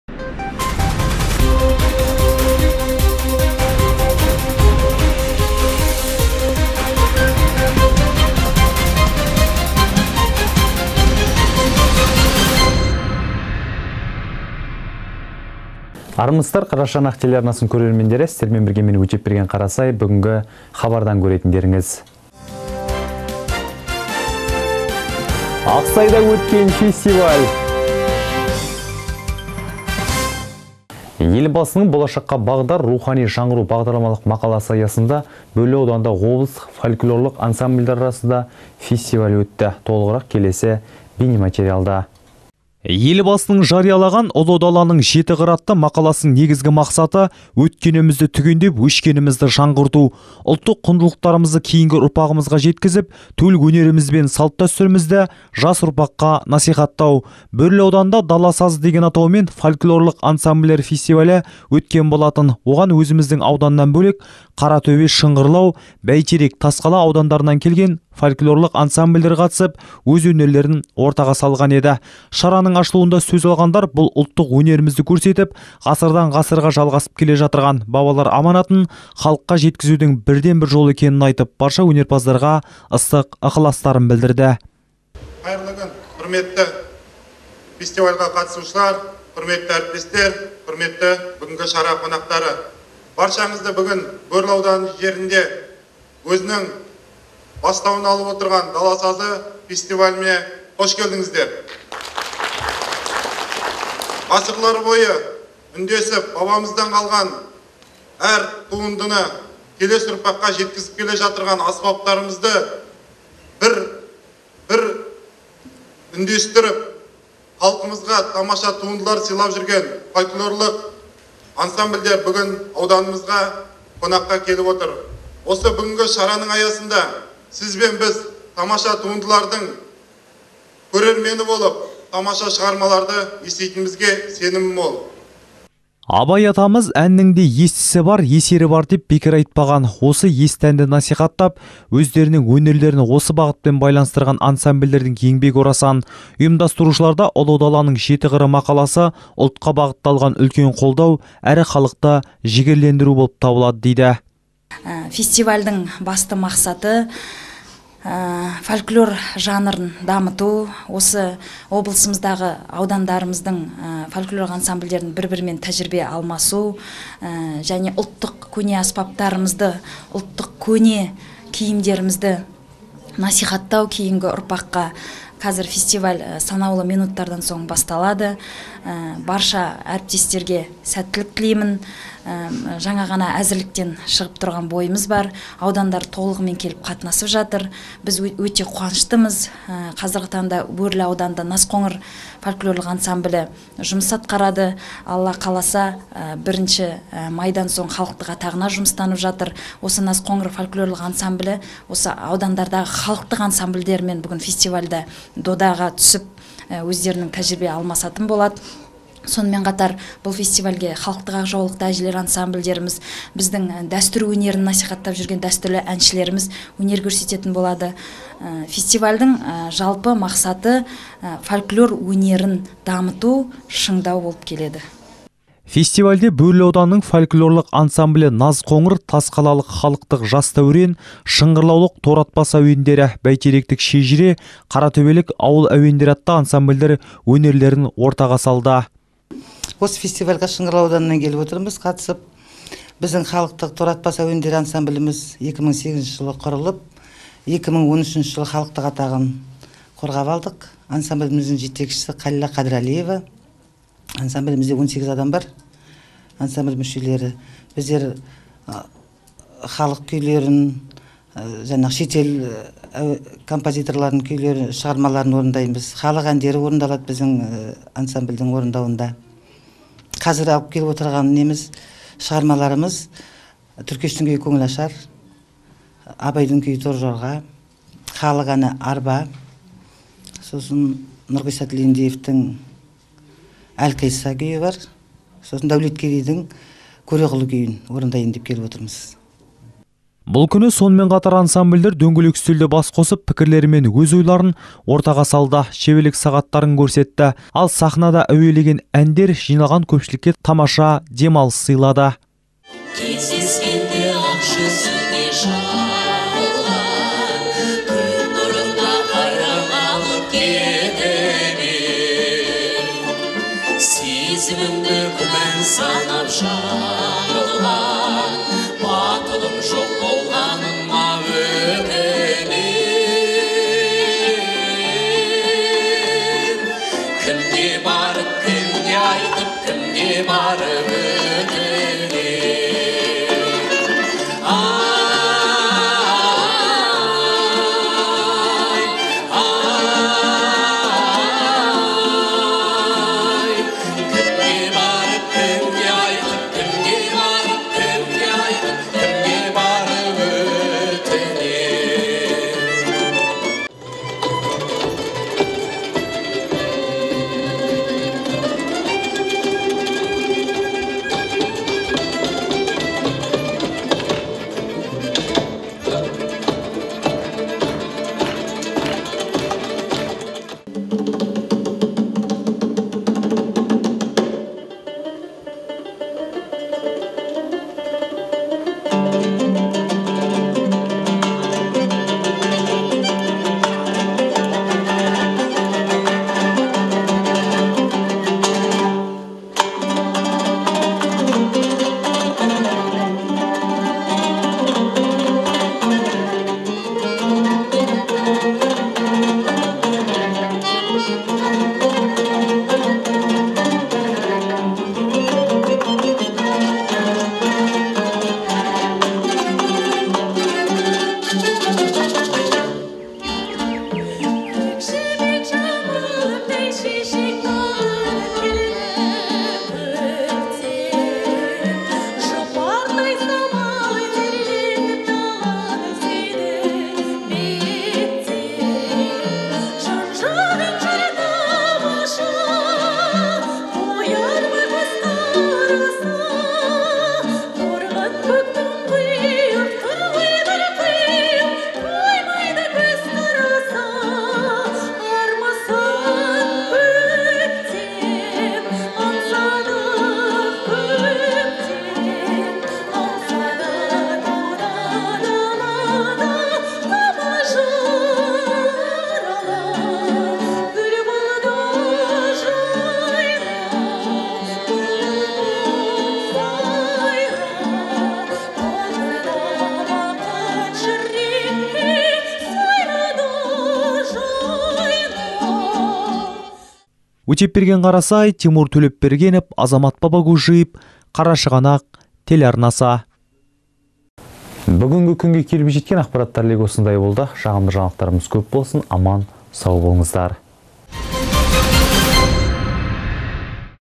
Ақсайда өткен фестиваль
Елбасының «Болашаққа бағдар; рухани жаңғыру» бағдарламалық мақаласы аясында Бөрлі ауданында облыстық фольклорлық ансамбльдер арасында фестиваль өтті. Толығырақ келесі бейнематериалда.